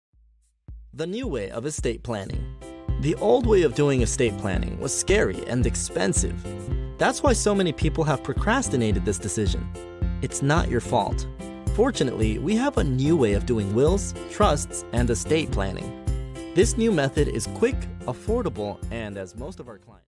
The New Way of Estate Planning
The Old Way of Estate Planning is Dead.
Try the New Way of Estate Planning.

How to avoid probate in Texas.
Make sure your money goes to the right people at the right time instead of to the wrong people or at the wrong time.
$0 Quick Consults available for Texas estate planning, probate & personal injury.

Office Address:
Mike Massey Law